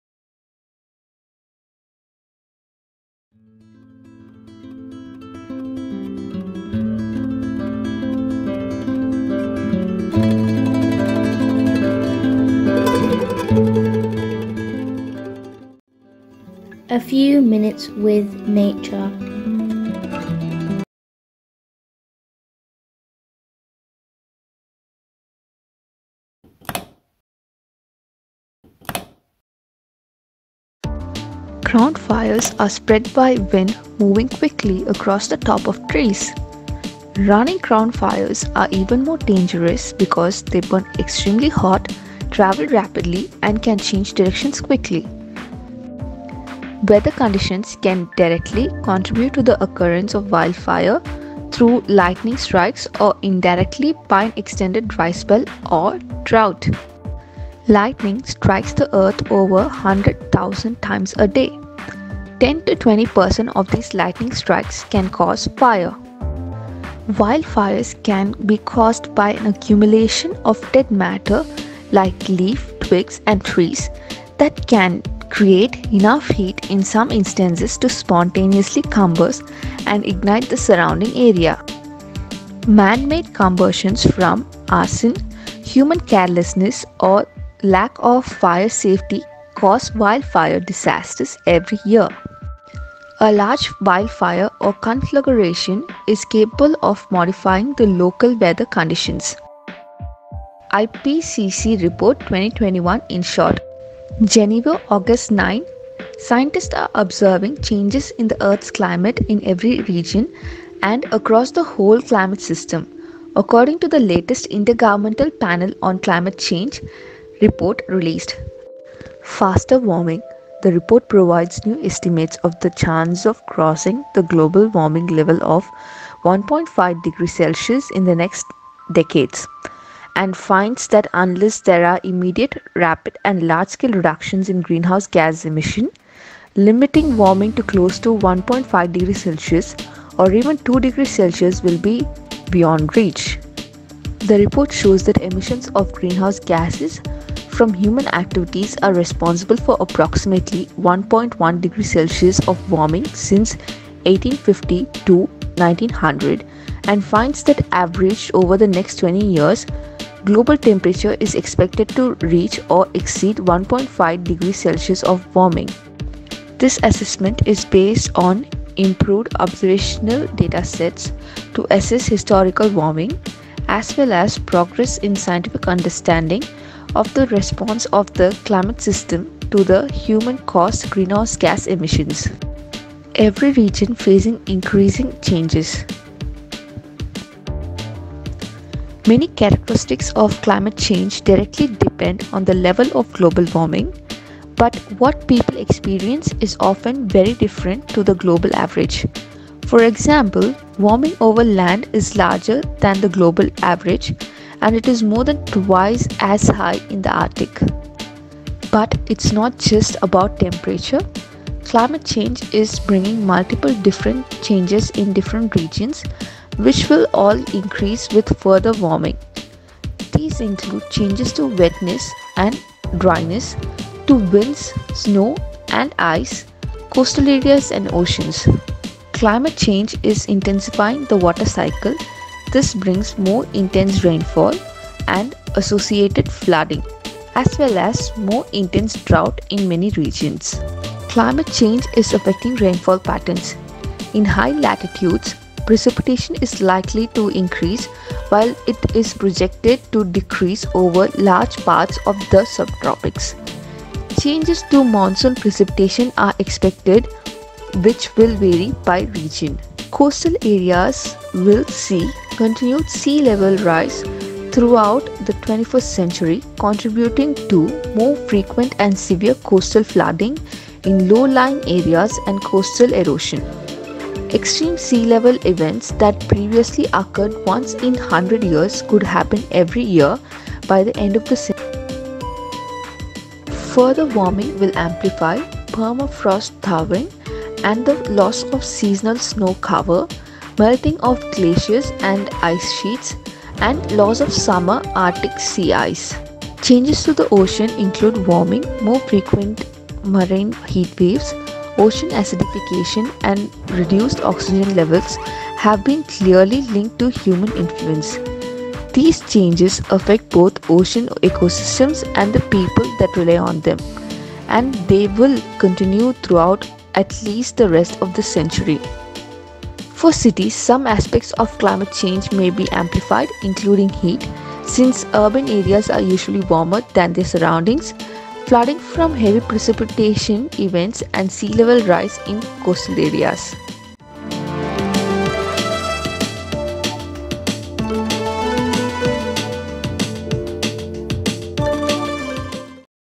Universal earth's transformation| Wildfire | Effects of Climate Change | Global warming
This video explains wildfire climate change & Global warming. The diversity of our planet is endangered by climate change. Climate change is a fundamental threat to people's livelihoods. Hope you will  get information.

Here are some more Nature related and Sleeping music videos.